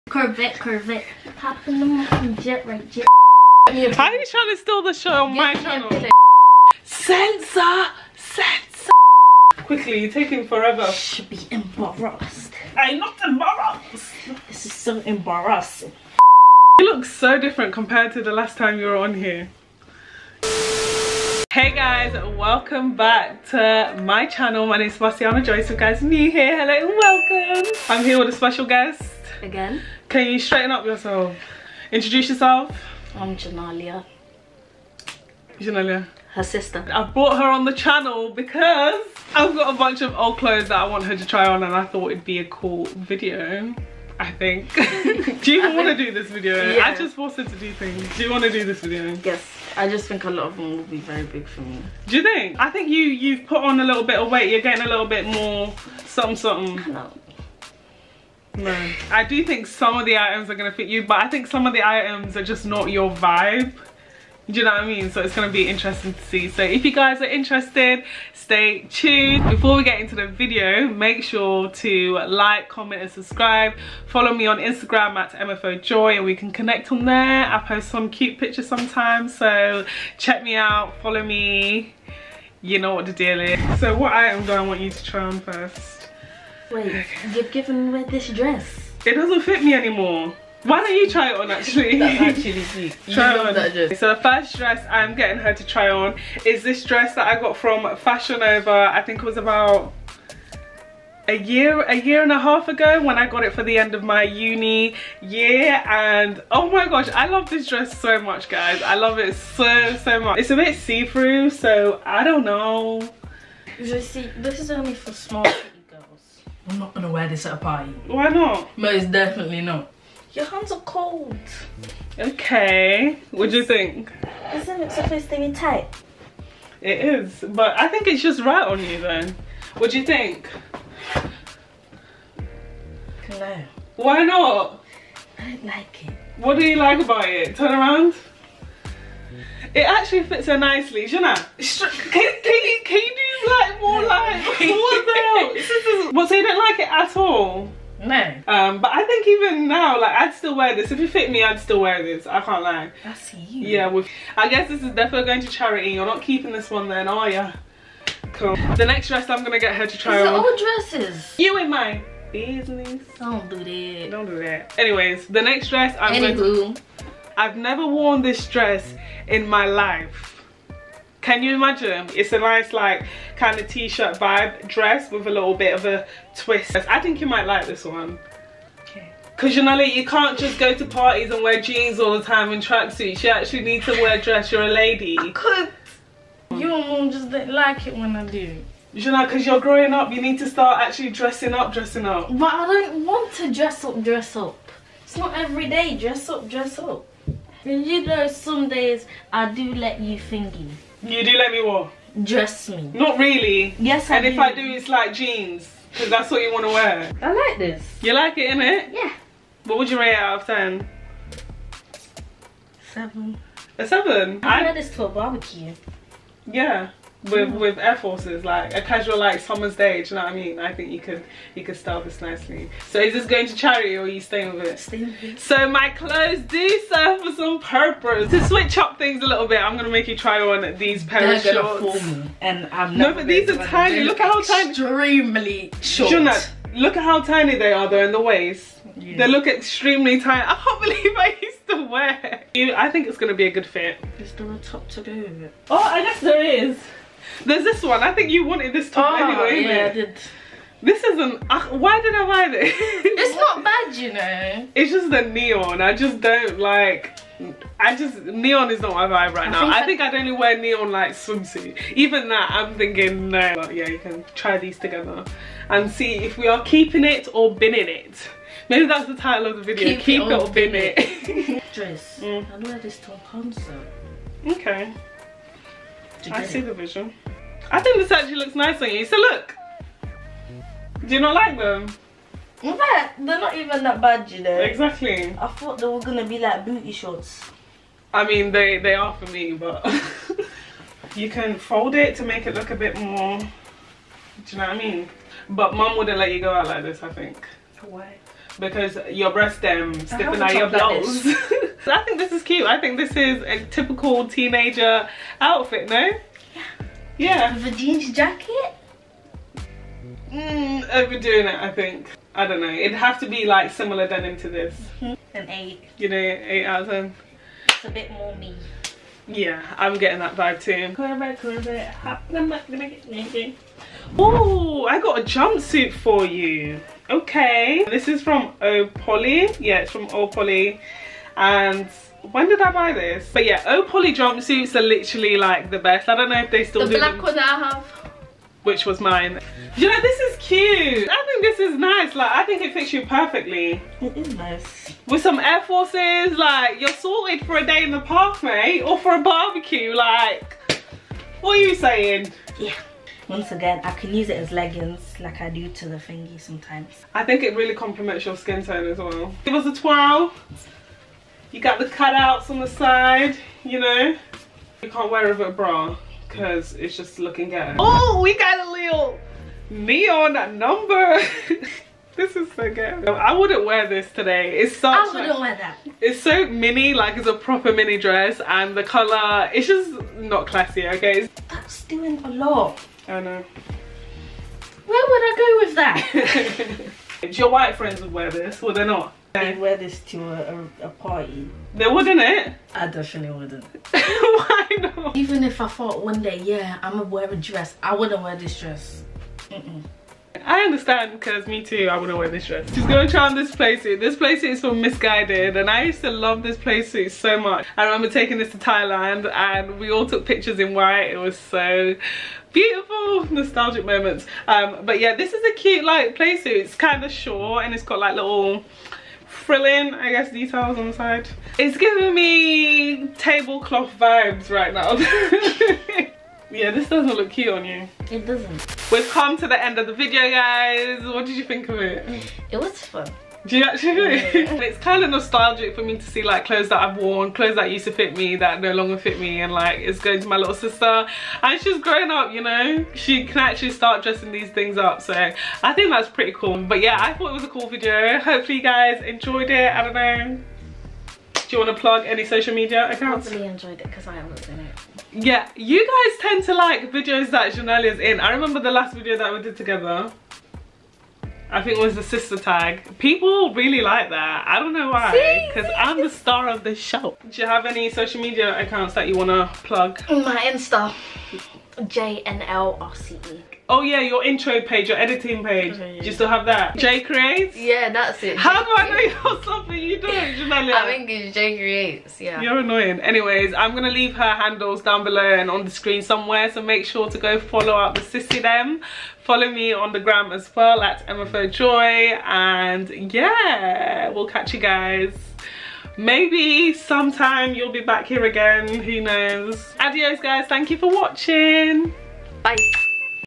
LITTLE SISTER TRIES ON MY OLD CLOTHES | SEBASTIANA JOYCE
Hey friends! This week's video i brought in my little sister to try on my old clothes lol. I thought it would be a cool idea and quite nostalgic too.. we had fun so I hope you like it as much as we did. Happy watching x

JUNALIA'S INSTAGRAMS: @JNLRCE AND @JAY._CREATES